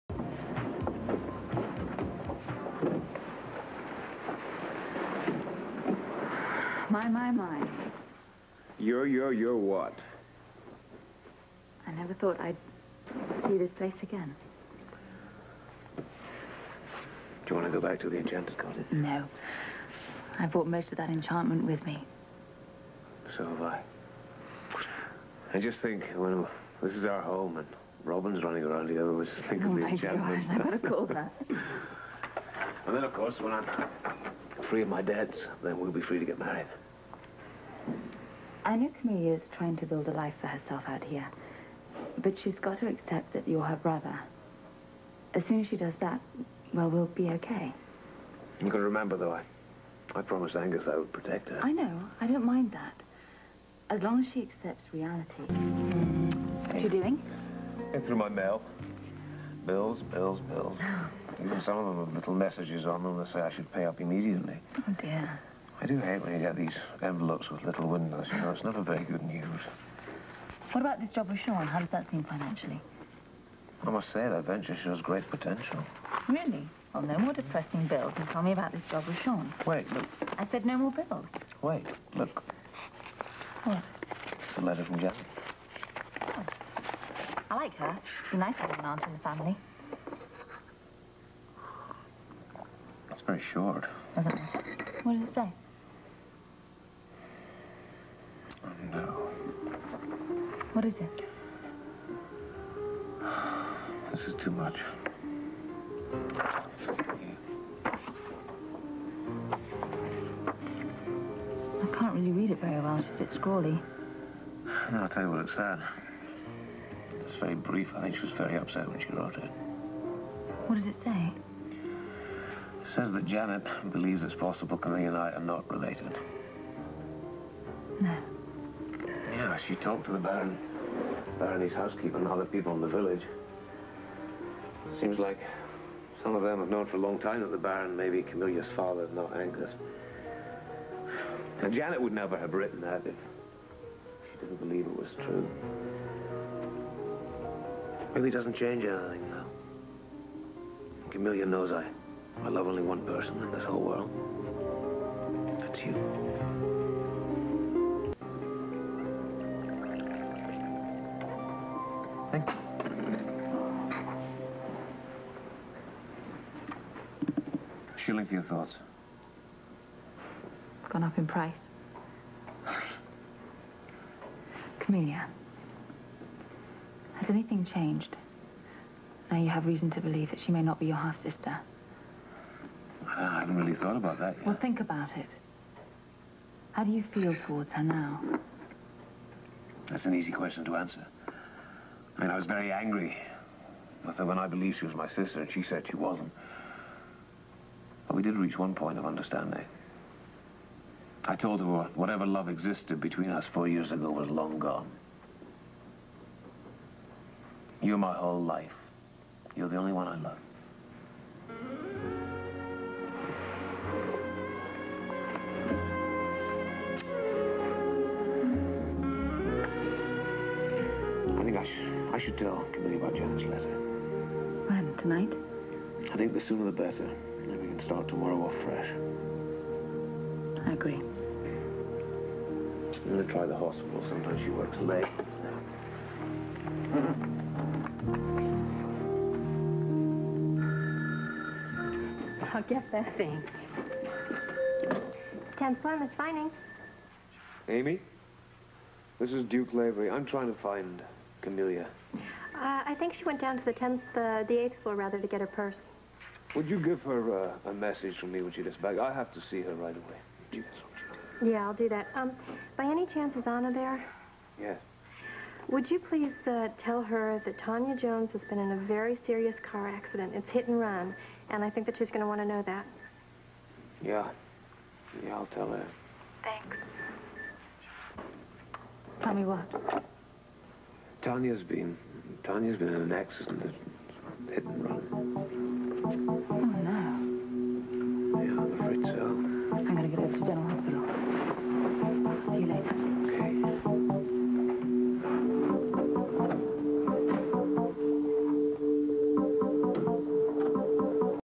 OnThisDay March 18, 1987
Anna and Duke return from the cottage. Duke receives a letter from his aunt. After some digging, Janet has learned that Camellia may be telling the truth about her true parentage.